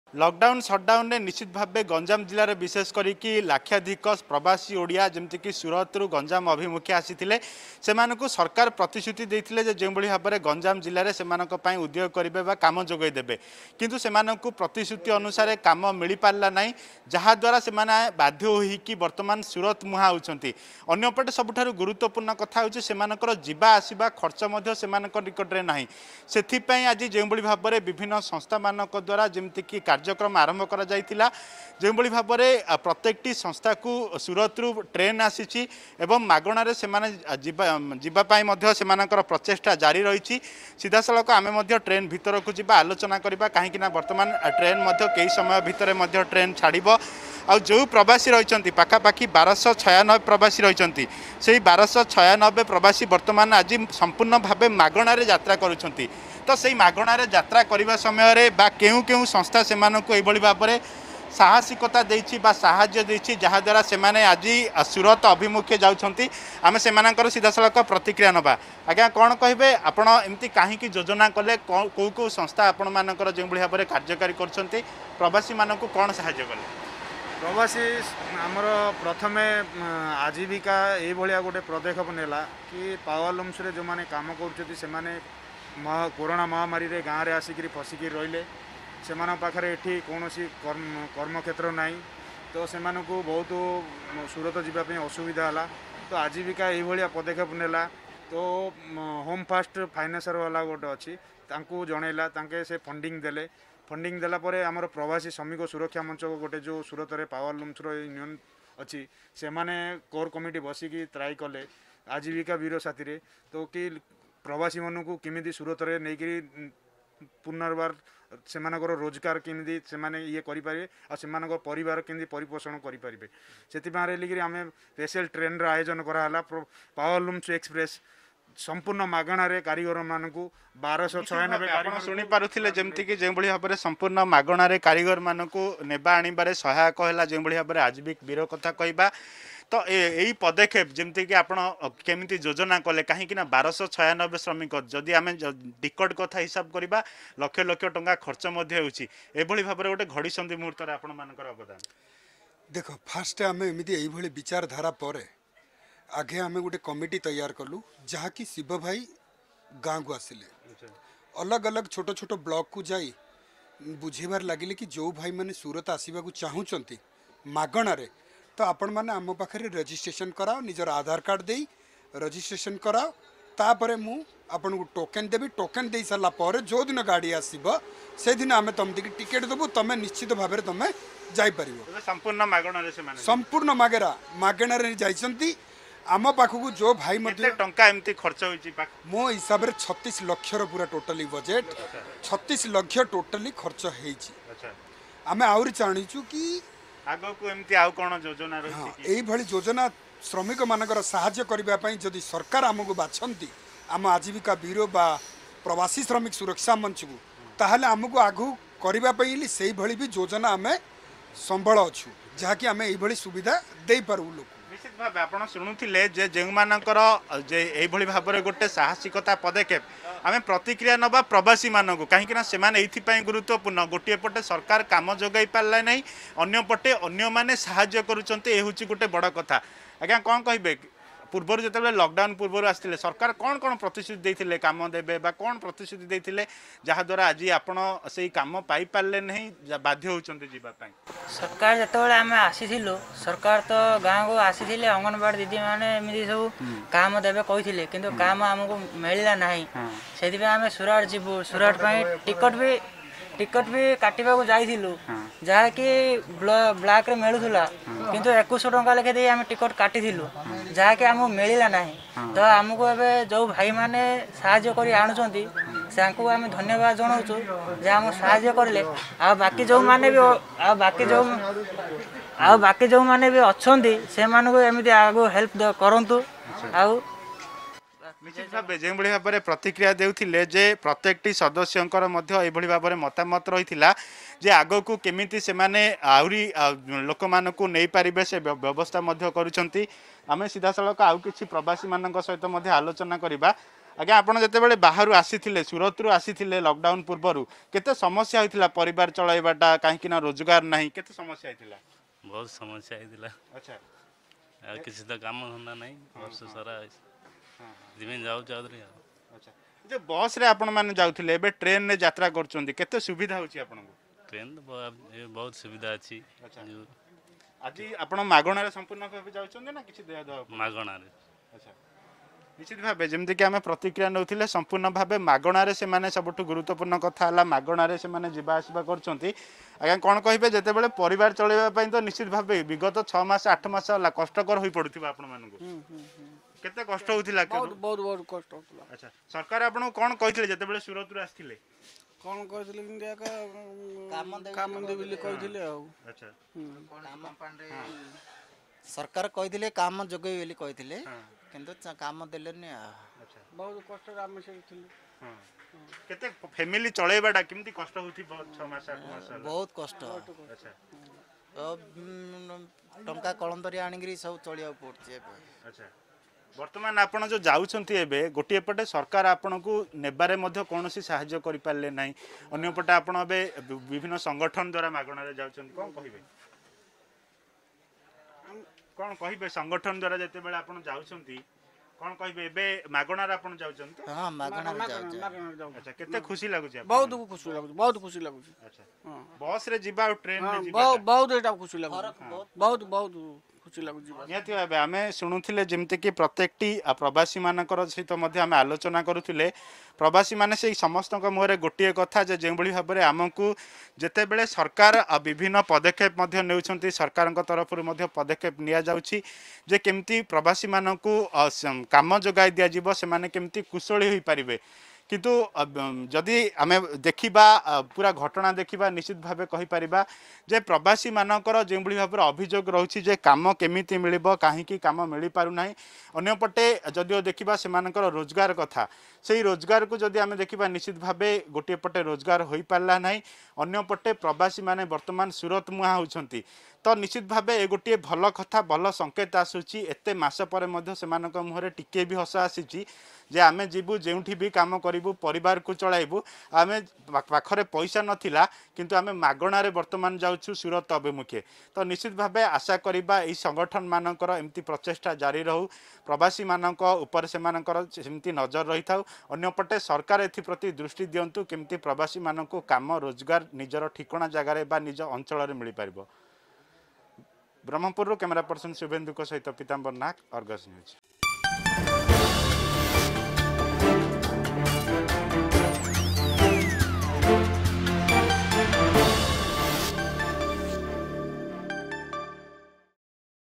ସମ୍ପୂର୍ଣ୍ଣ ମାଗଣା ରେ 1296 ପ୍ରବାସୀ ବ୍ରହ୍ମପୁର ରୁ ସୁରତ ବାହାରିଲେ
ରାଜ୍ୟ ସରକାର କାମ ଦେଲେନି, ପୁଣି ରାଜ୍ୟ ବାହାରକୁ ଯାଉଛନ୍ତି ହଜାର ହଜାର ପ୍ରବାସୀ।ତେବେ କେମିତି ମାଗଣା ରେ ଯିବା ସମ୍ଭବ ହେଲା.